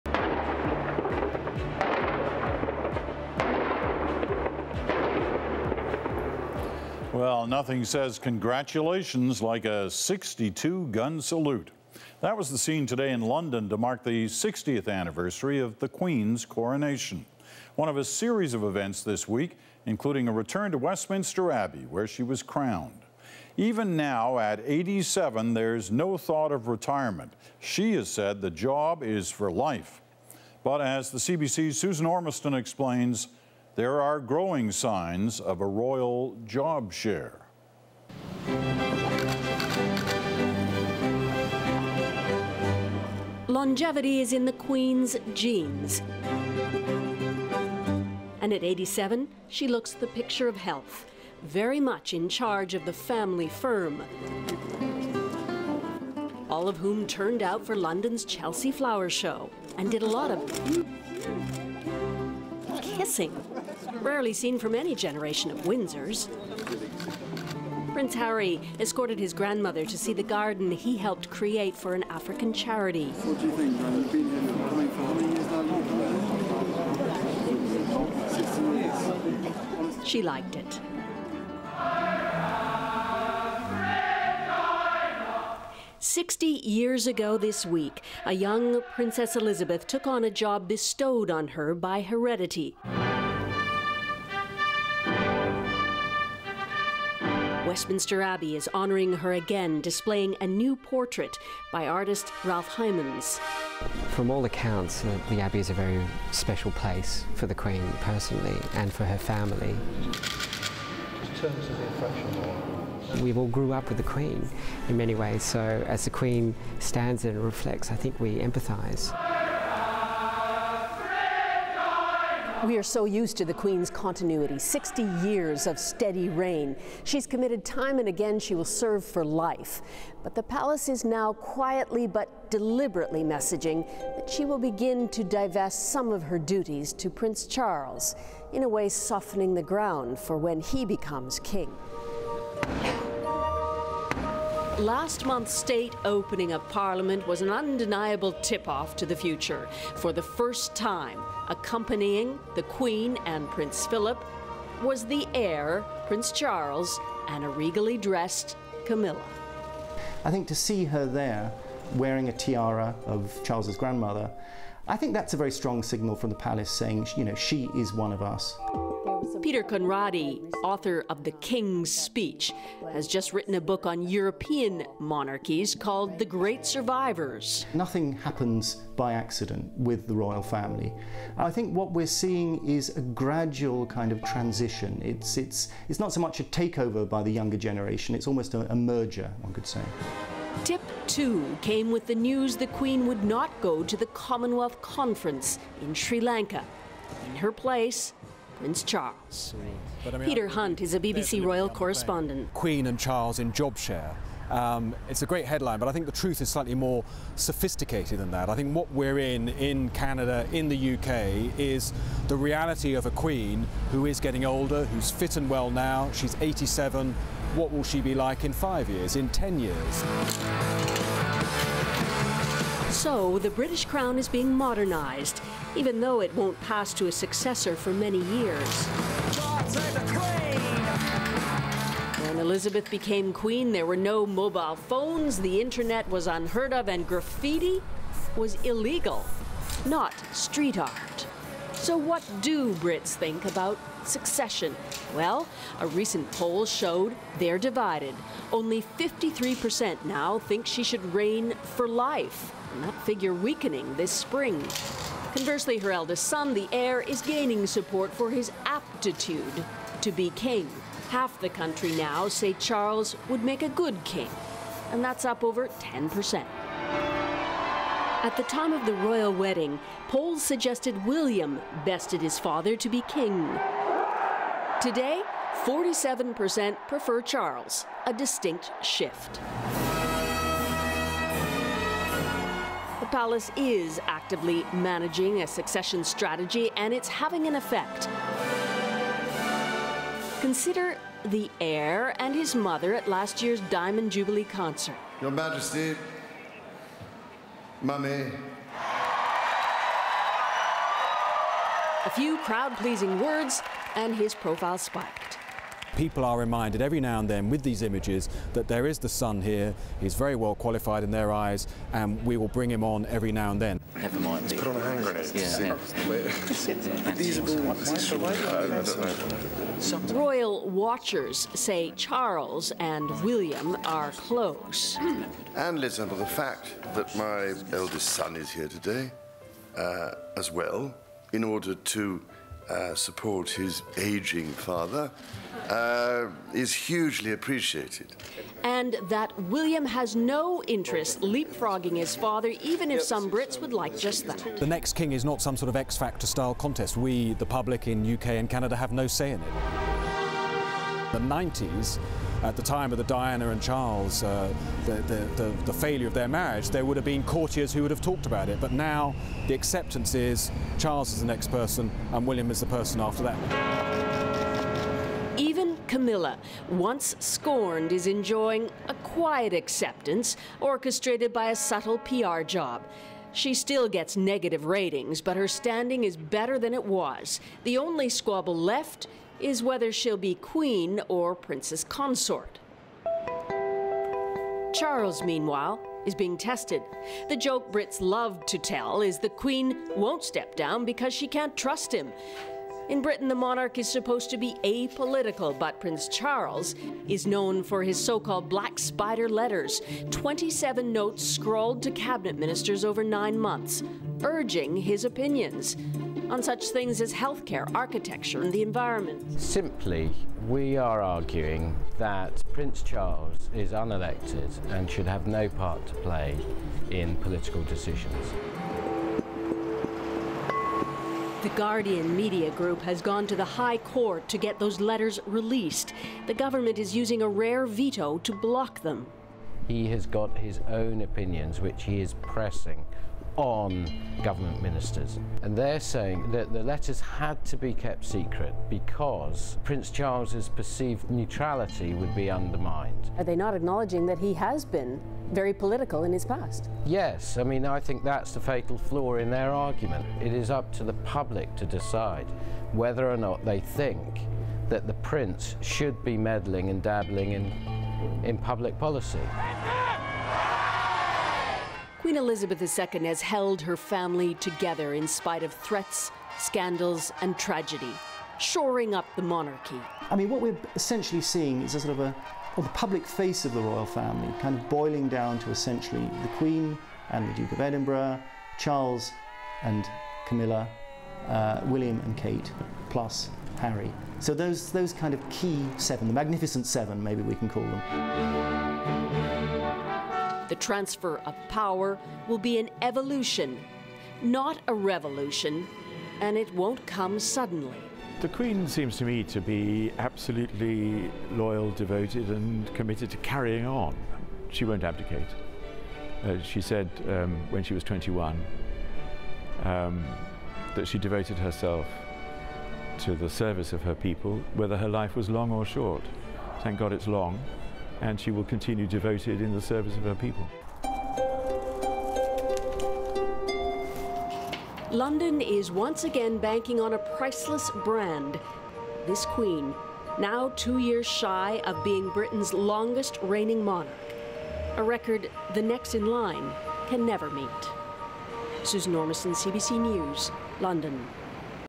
A Job for Life
As Queen Elizabeth celebrates 60 years on the throne, Susan Ormiston looks at the subtle campaign to break in the next British monarch.